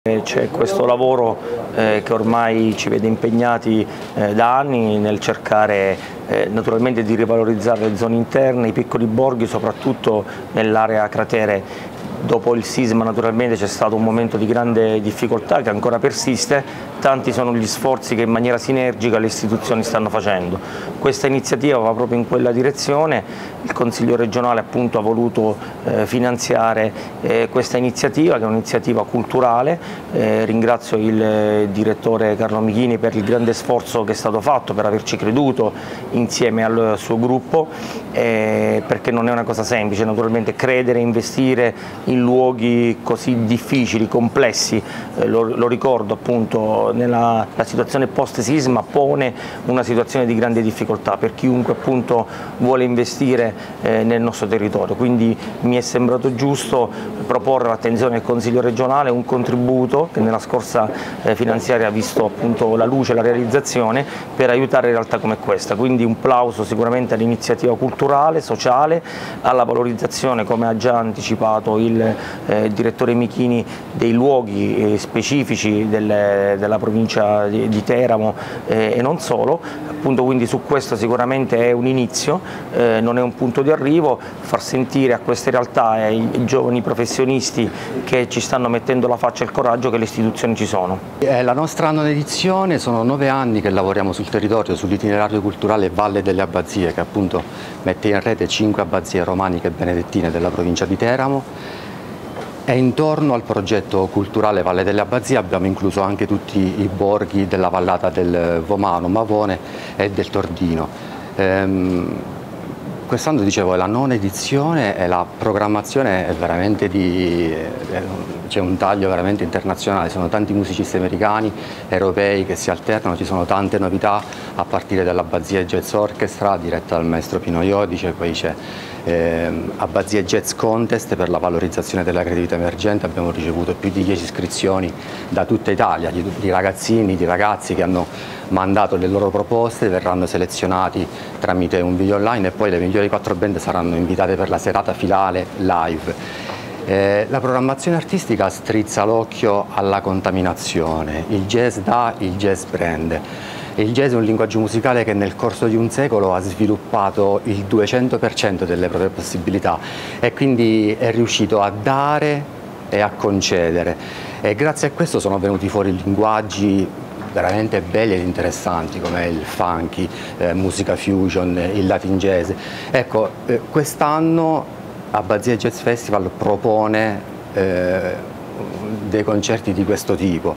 Mariani alla presentazione di Abbazie Jazz Festival
Il consigliere regionale Sandro Mariani ha partecipato alla conferenza stampa di presentazione dell'edizione 2023 di Abbazie jazz Festival, manifestazione che ha ricevuto un contributo del Consiglio regionale, in programma dal 17 luglio all'11 agosto in varie location della Valle delle Abbazie tra i fiumi Vomano e Mavone nel teramano.
🗣 Le dichiarazioni di Mariani e del direttore artistico Carlo Michini